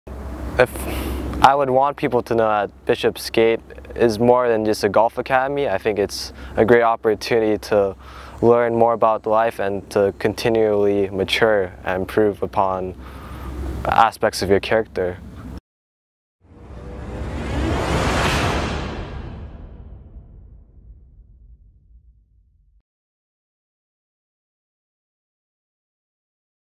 Testimonial: Greg Shen on BGGA
BGGA student Greg Shen shares the impact BGGA has had on his life.